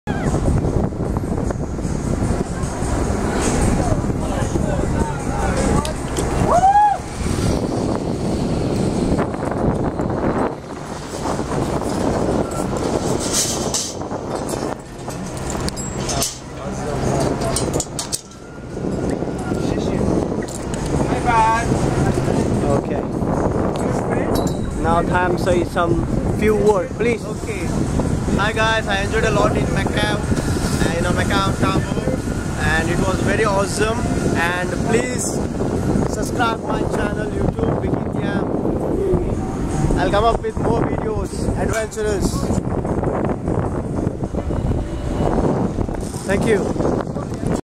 World's Highest Bungy Jump & Sky walk - Macau Tower Bungee - AJ Hackett | TA VLOG
World's Biggest & Highest Bungee Jump , sky walk at the Macau Tower. On our recent trip to Hong Kong vikith m visited the AJ Hackett Macau Tower Bungy and sky walk off the ledge to the safety mat 233 meters (764 feet) below!